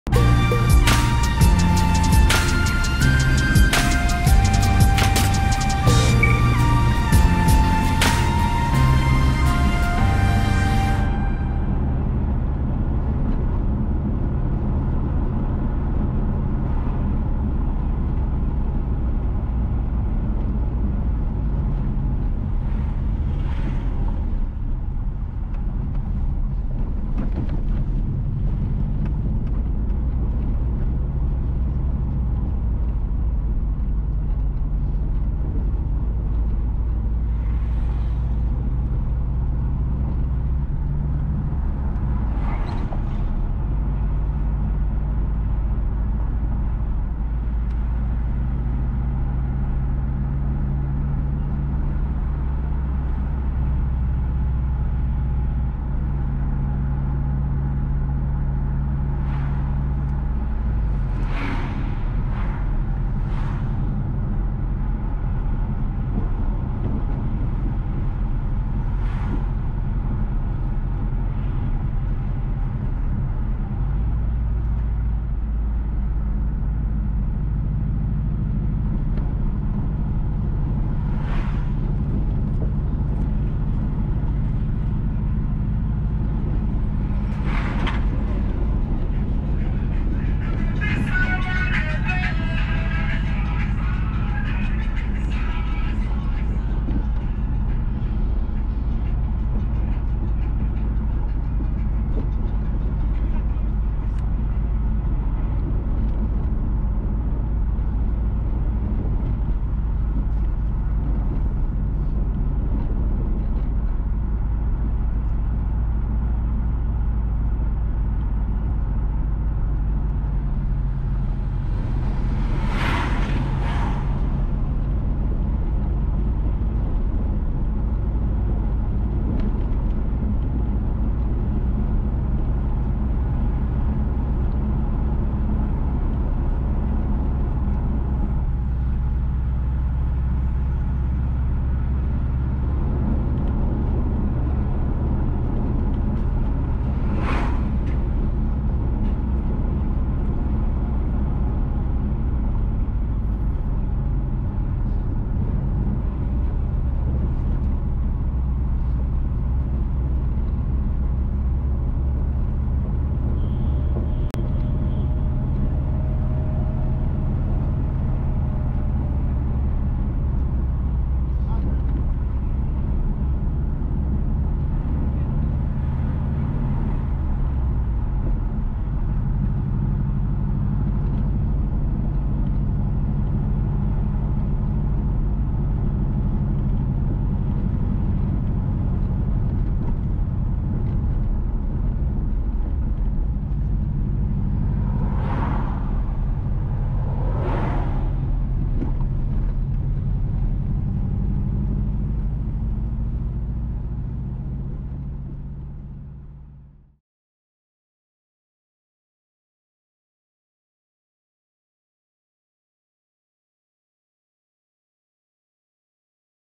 Cobbla, Manchester, Jamaica
Cobbla is located in Manchester, Jamaica (Caribbean).

⭐ Points of interest:
0:00 Intro
00:12 Clandon
00:17 Entering Cobbla
00:41 Knox Community College, Cobbla
01:28 Cobbla New Testament Church Of God
02:00 To Cobbla Cliff Holes (Cave)
02:07 Church Of God Seventh Day Cobbla
02:48 To Pentecostal Church of God (Trinity), Village
03:06 Entering Mount Olivet

LOCATION:
2020 Jamaica Road Trip